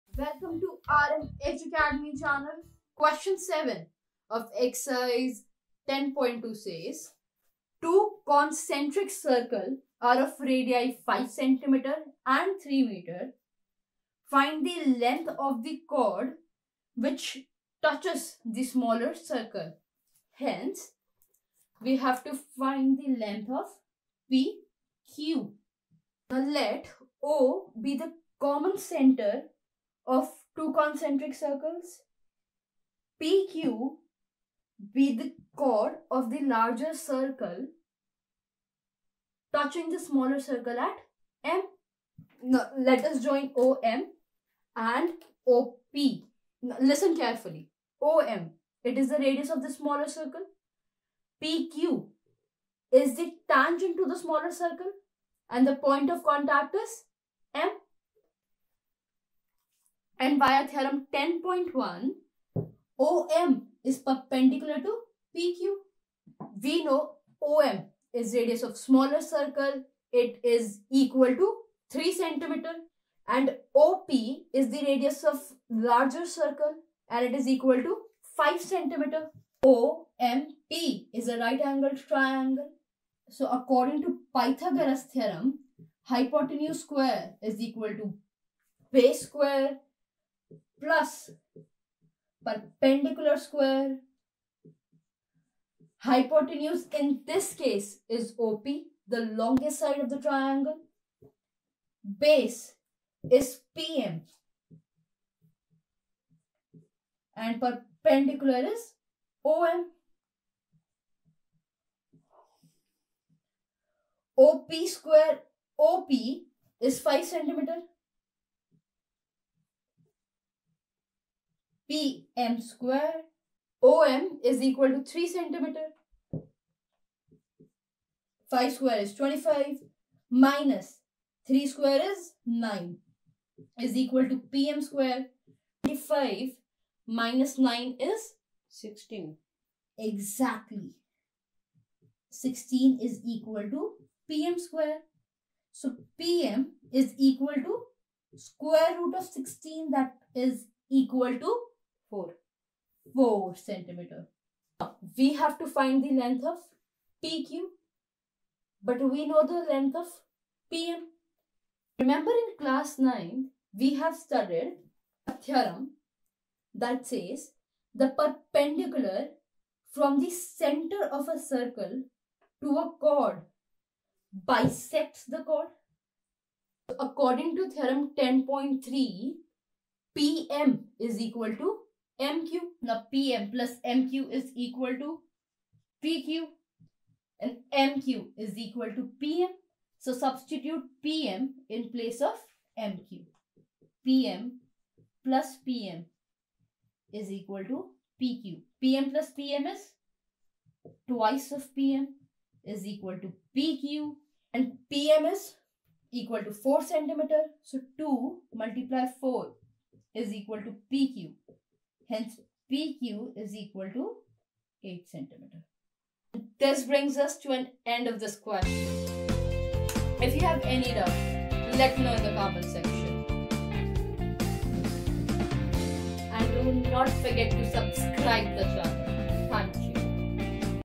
Two concentric circles are of radii 5 cm and 3 cm | Exercise 10.2 [Q7]
Exercise 10.2 Question 7 Class 10
Two concentric circles are of radii 5 cm and 3 cm. Find the length of the chord of the larger circle which touches the smaller circle.

 HINT :
Theorem 10.1 :
The tangent at any point of a circle is perpendicular to the radius through the point of contact

 Theorem 10.3 : (Class 9)
 The perpendicular from the center of a circle to a chord bisects the chord.

Read it :
INTRODUCTION
A circle is a collection of all points in a plane which are at a constant distance (radius) from a fixed point (center).
Different situations that can arise when a circle and a line are given in a plane.
(i) The line PQ and the circle have no common point. In this case, PQ is called a non-intersecting line with respect to the circle.
(ii) There are two common points A and B that the line PQ and the circle have. PQ a secant of the circle.
(iii) There is only one point A which is common to the line PQ and the circle line is called a tangent to the circle.

Tangent to a Circle
A tangent* to a circle is a line that intersects the circle at only one point.
There is only one tangent at a point of the circle.
The tangent to a circle is a special case of the secant, when the two end points of its corresponding chord coincide.

The common point of the tangent and the circle is called the point of contact and the tangent is said to touch the circle at the common point

Note📝
The above question is taken from NCERT class 10 chapter Circles exercise 10.2 question 7.

Practice :
The length of a tangent from a point A at distance 5 cm from the center of the circle is 4 cm. Find the radius of the circle.